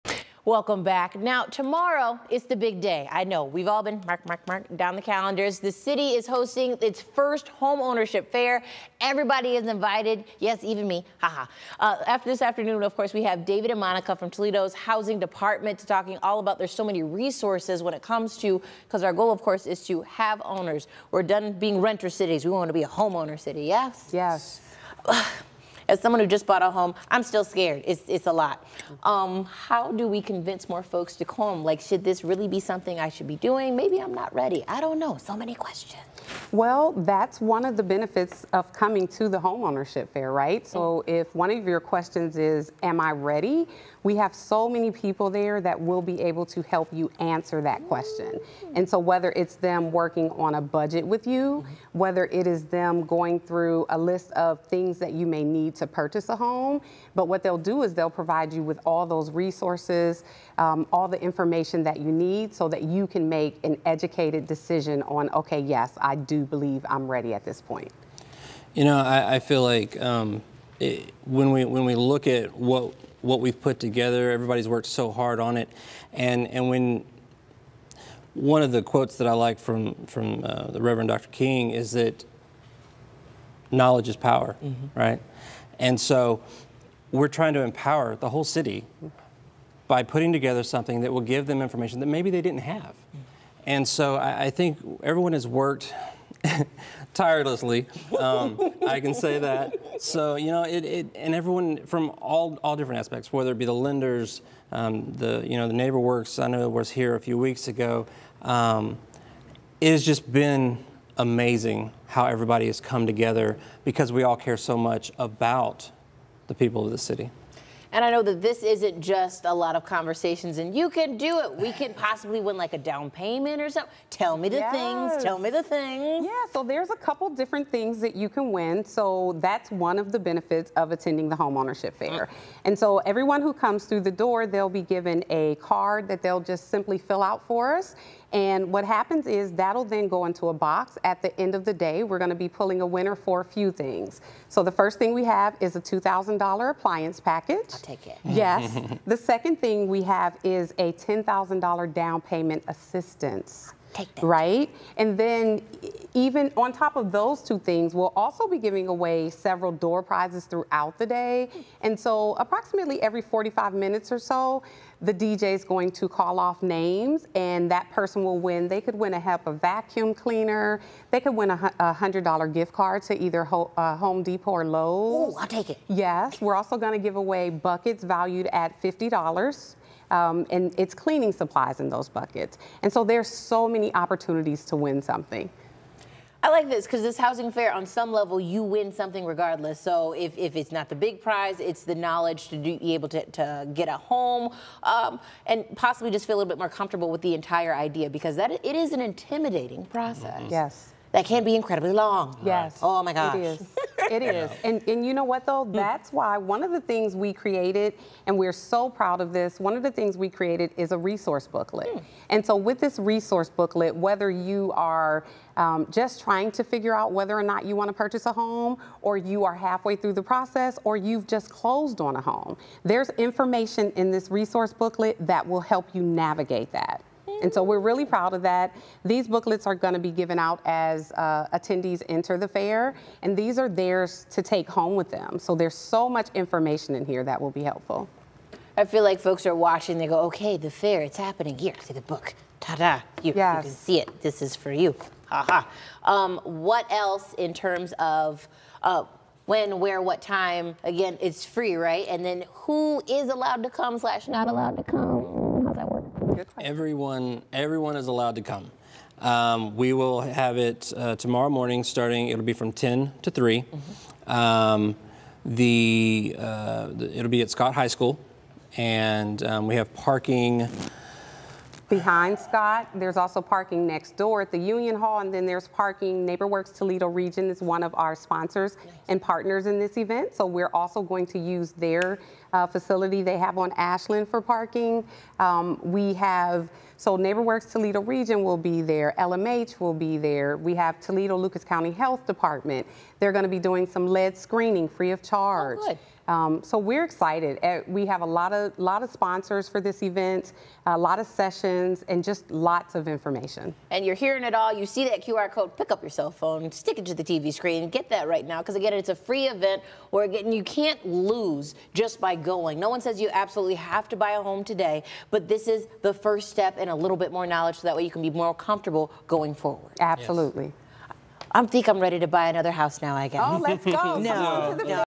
Homeownership Fair aims to provide Toledoans with resources, knowledge to prepare for the next step
In addition to experts answering questions, there will also be chances to win door prizes. The Homeownership Fair is June 22 at Scott High School from 10 a.m.-3 p.m.